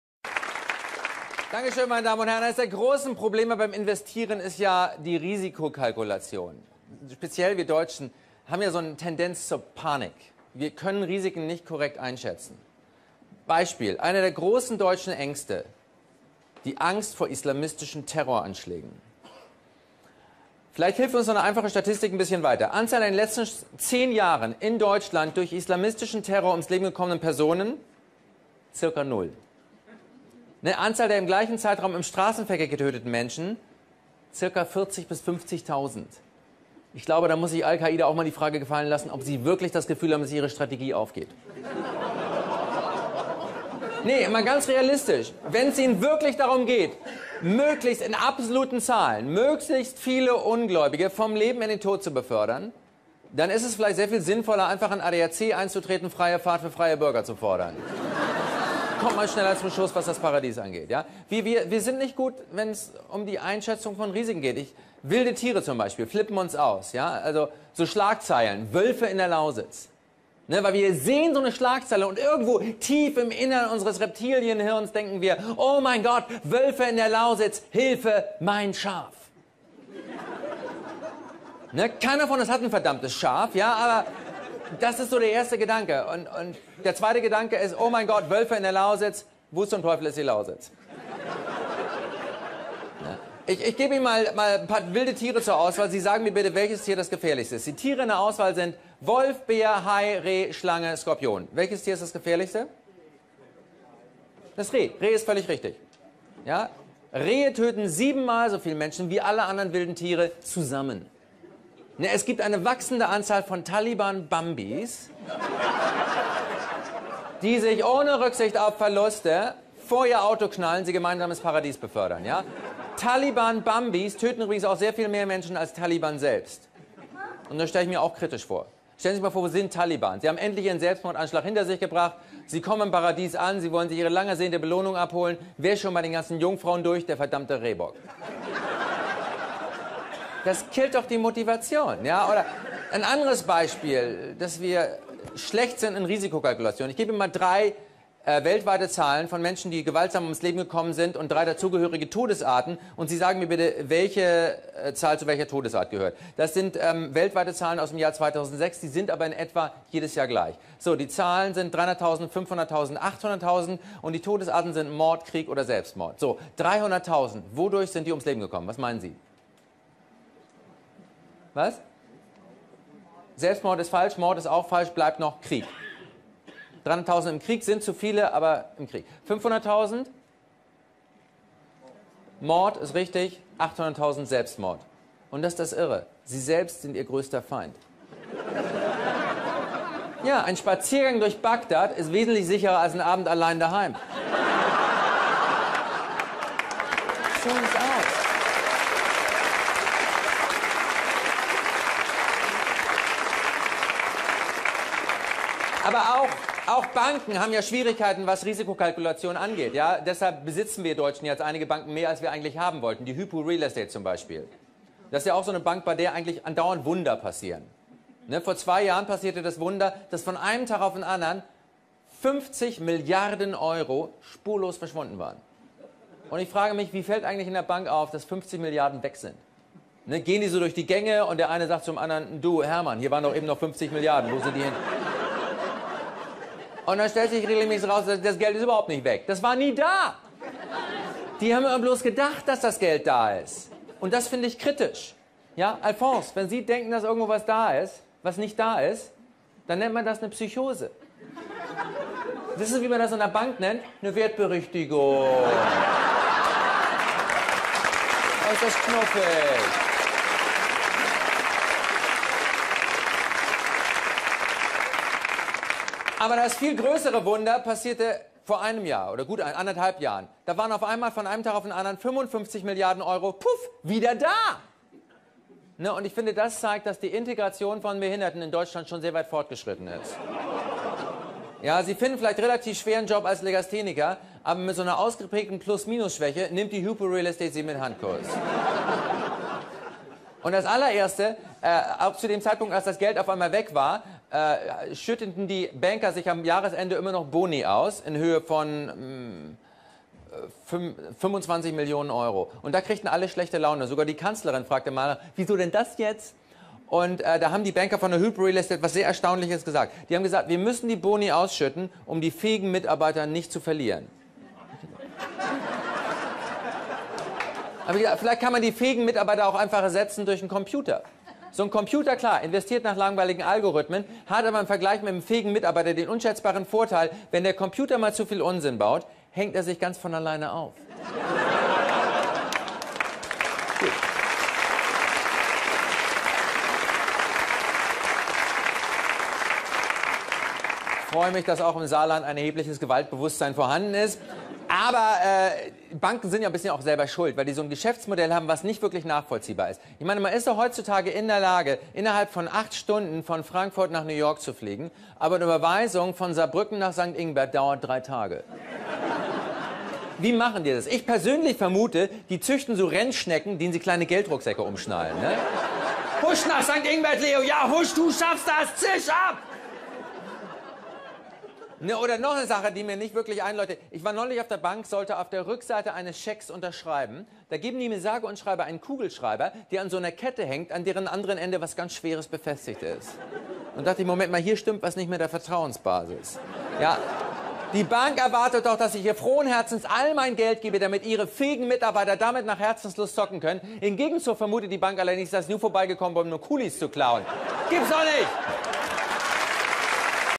BANKSTER WIE 50 Milliarden VERSCHWINDEN ... Comedy
Politiksatire - comedy über Banker , Verbrecher und sonstige Geschwüre..